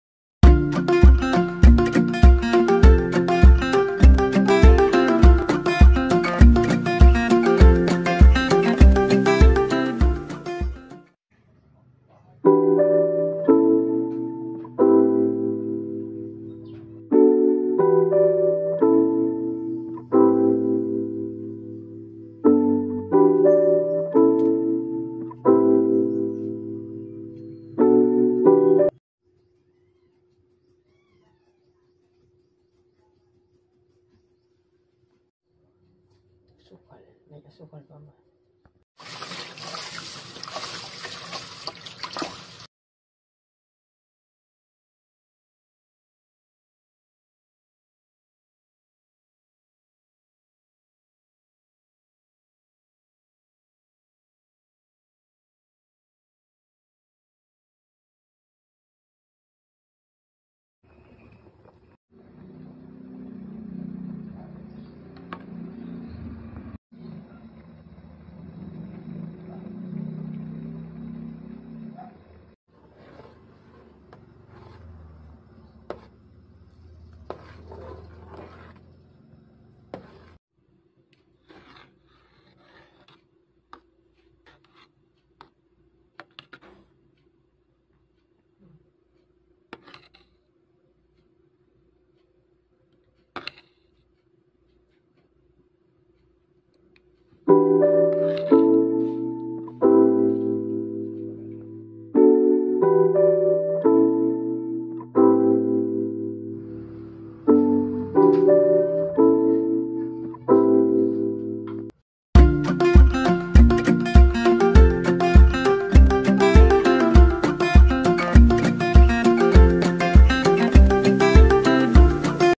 Easy UBE Champorado Recipe| 2 ingredients only| Barefoot in the kitchen
Magandang umaga!
Gagawa tayo ng instant Champorado with a twist.
Watch and make your own version.